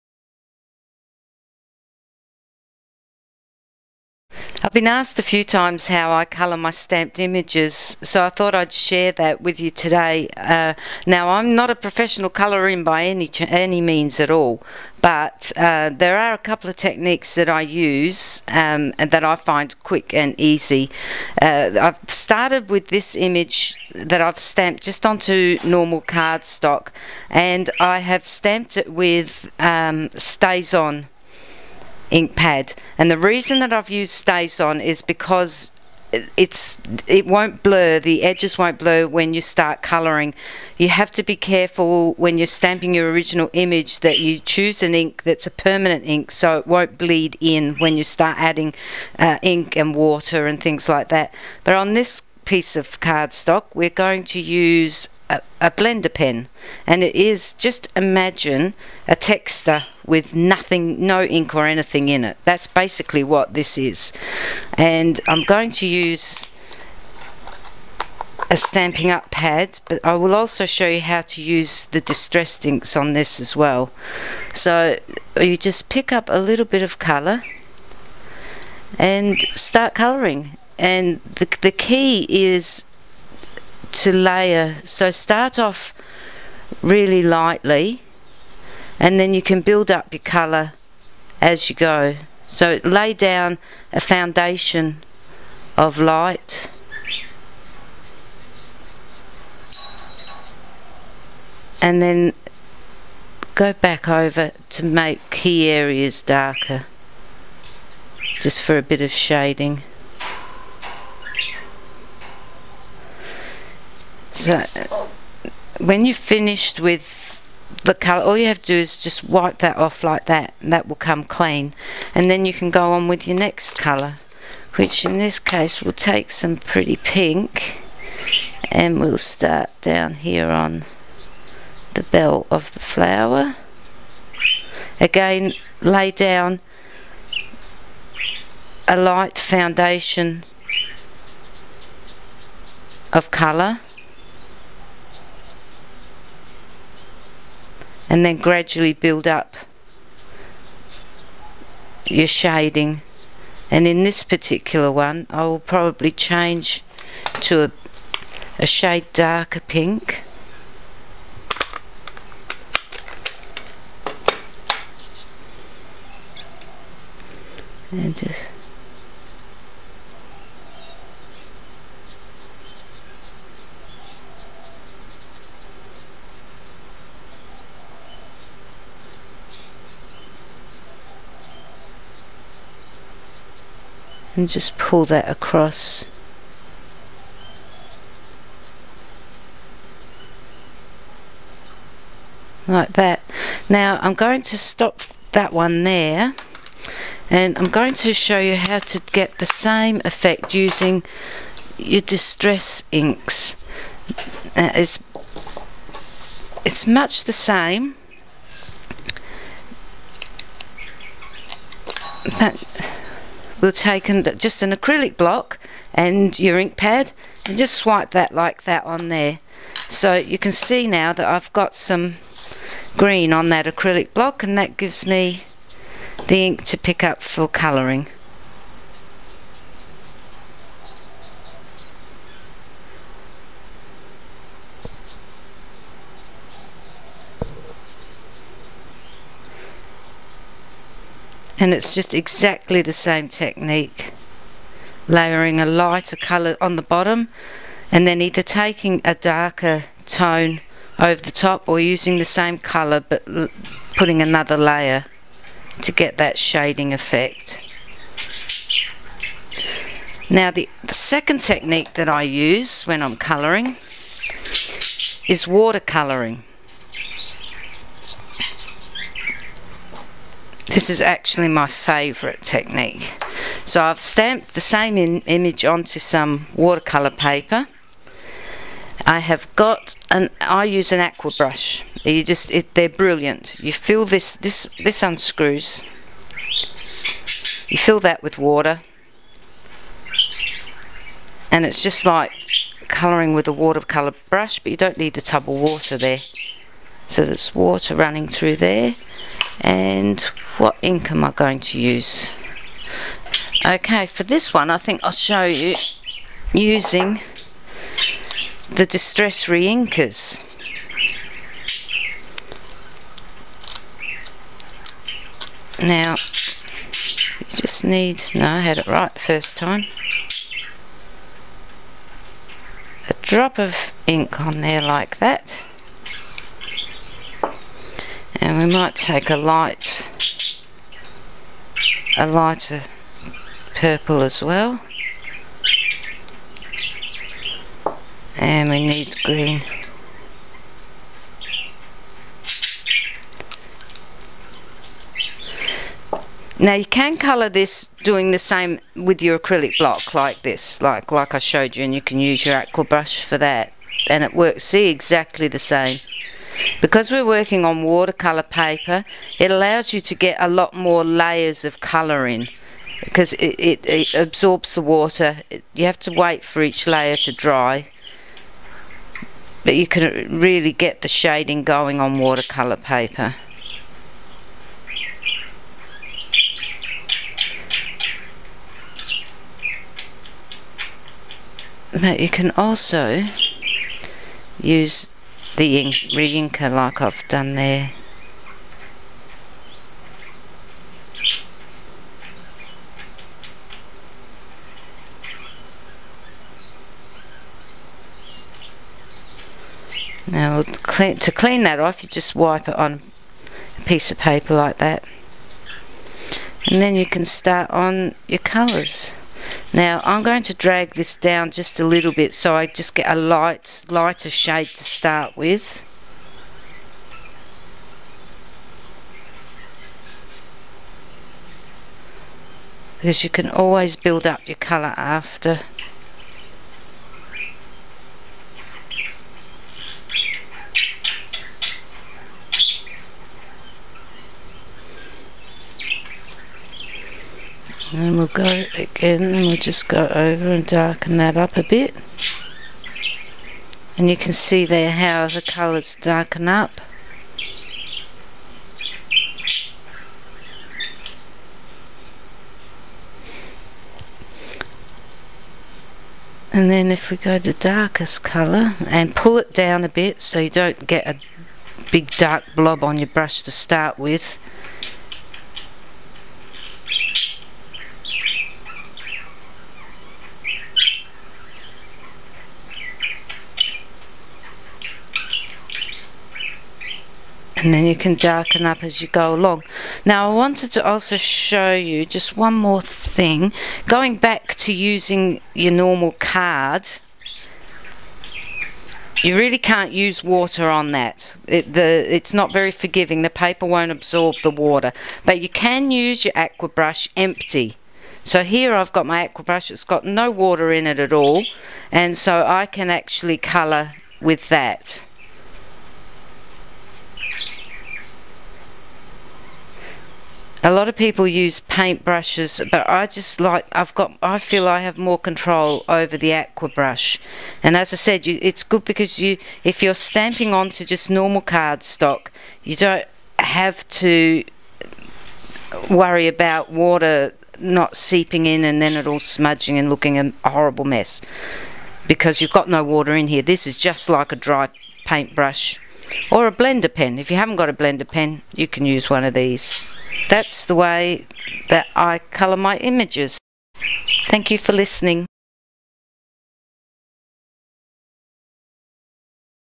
Colouring Stamped Images
Helen shows you 3 different techniques for colouring stamped images using Stamp'in' up ink pads, distress inks, a bender pen and an aqua brush